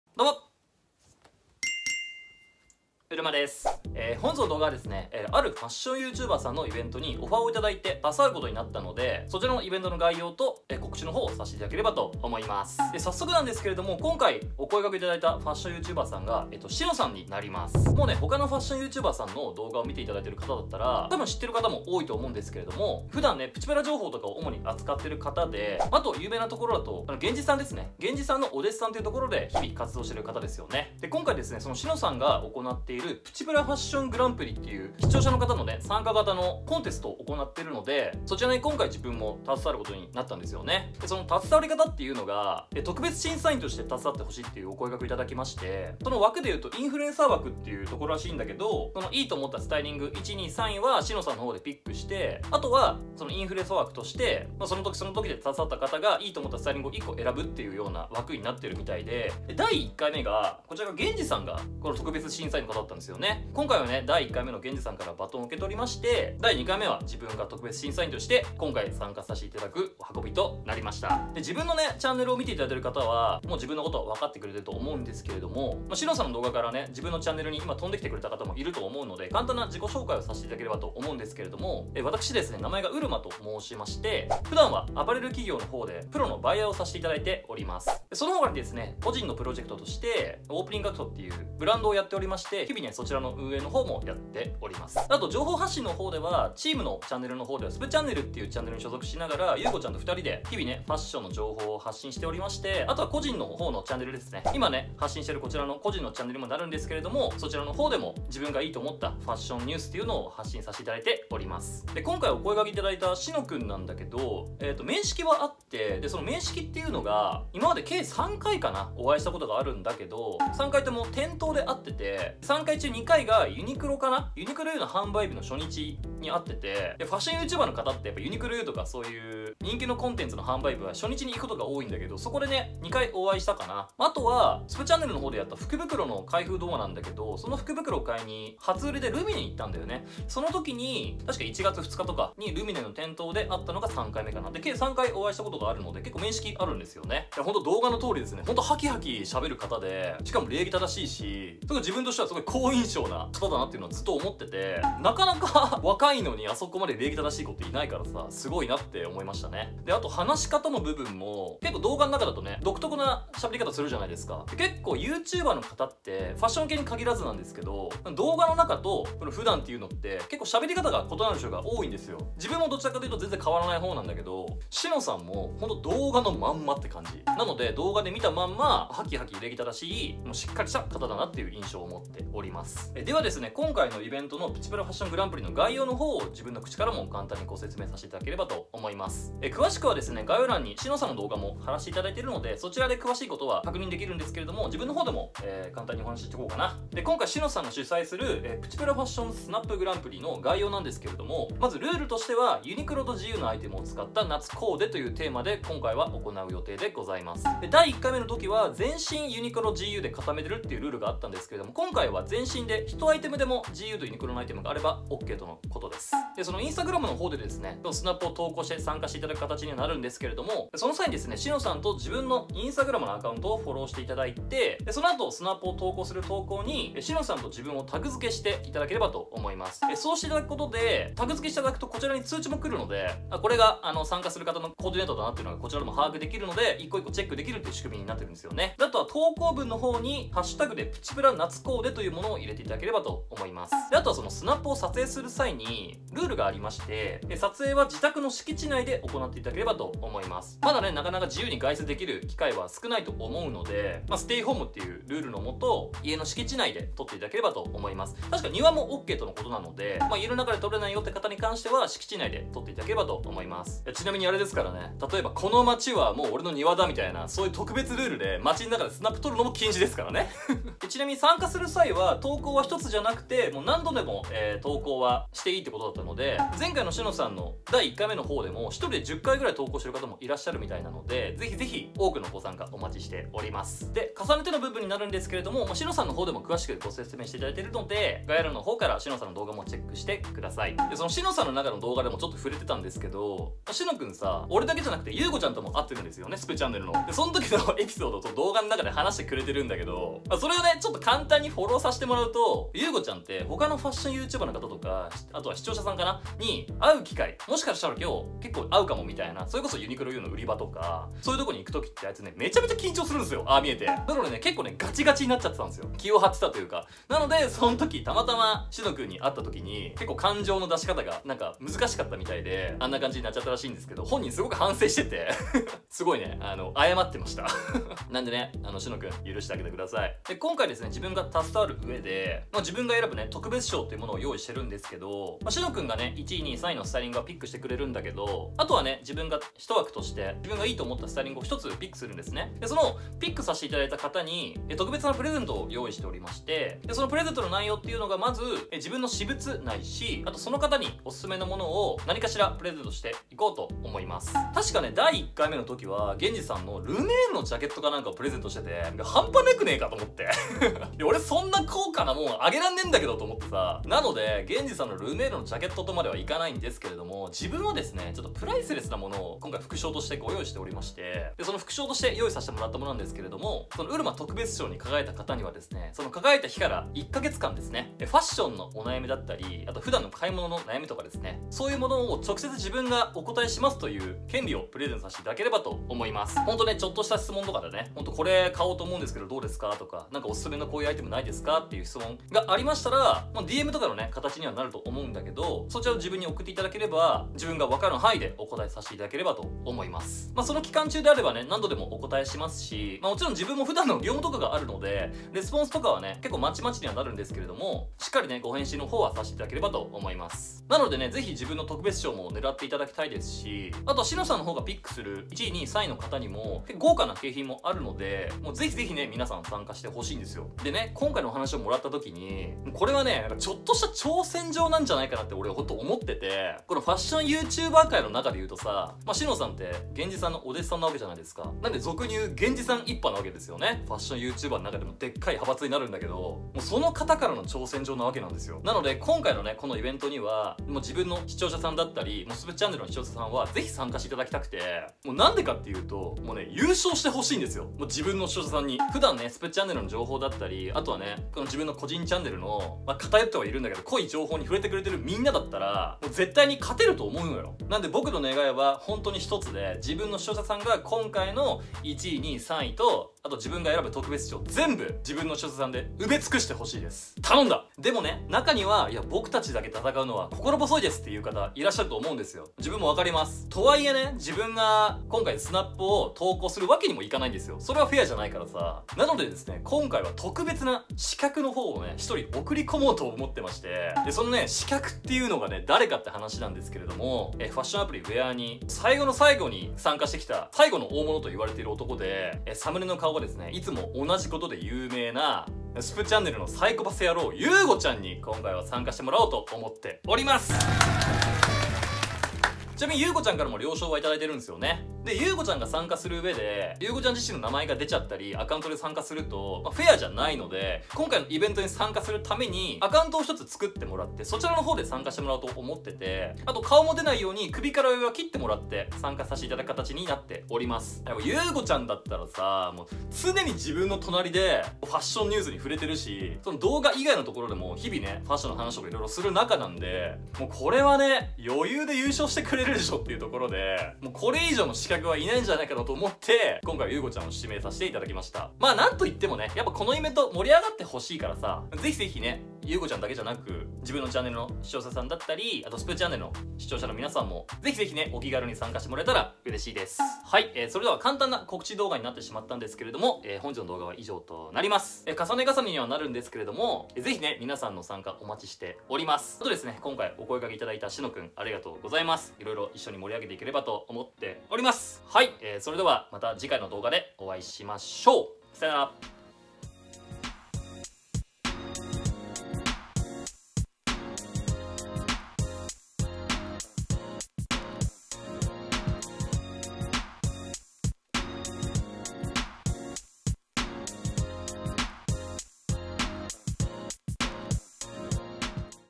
【げんじ一派からの挑戦状!?】ファッションYouTuberさん主催のプチプラスナップグランプリに参加することになったので、詳細をお話します！【 しのさん / げんじさん GU / ユニクロ 】
■しのさんプチプラファッショングランプリの告知動画はこちら!!

今回ファッションYouTube界のキワ者の俺にお声かけ頂き、ありがとうございます！笑

お声かけ頂いたので、ばっちり一緒に盛り上げていけたらと思います！

さて、

いつも俺の動画やスプチャンネルを見てくれてるみんな！

動画の中でも言ったけど、

絶対勝ってくれ！すべてのランキングを埋められるようにゆうごちゃんと共に頼んだ！

■5月31日(日)21:00～販売開始のアイテムラインナップはこちらから!!

■ウルマ∞ブログ【ファッションレジスタンス活動記】

■ウルマ∞プロデュースオンラインショップ
【OPENING ACT】